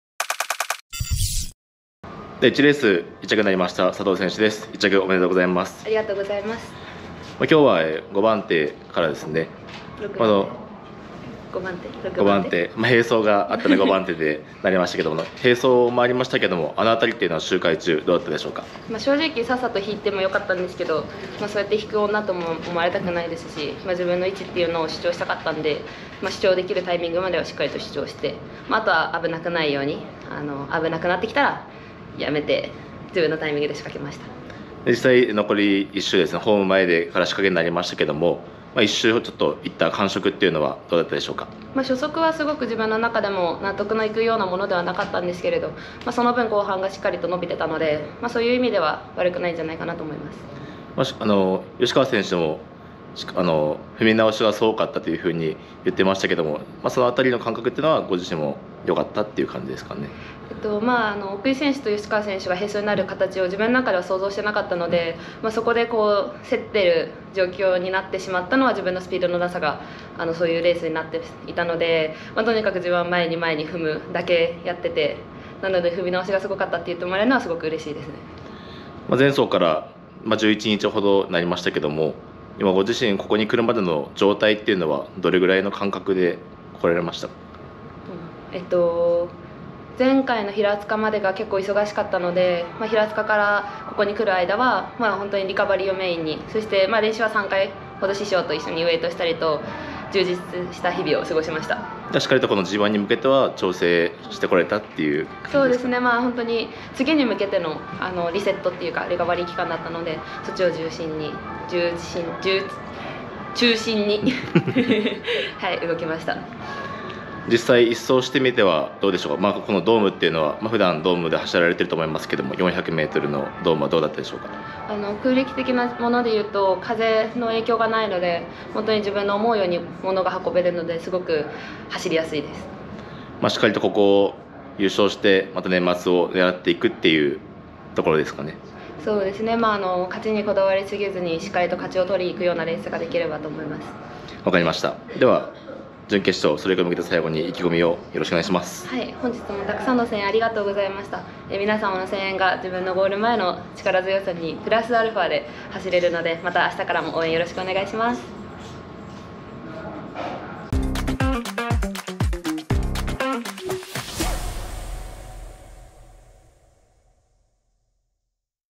小倉競輪競輪祭女子王座戦GⅠナイター 佐藤水菜(神奈川・114期)初日1R L級ガールズ予選 1着
トリマクリでは小倉競輪競輪祭・競輪祭女子王座戦GⅠナイターのSPECIALCONTENTSを開設中！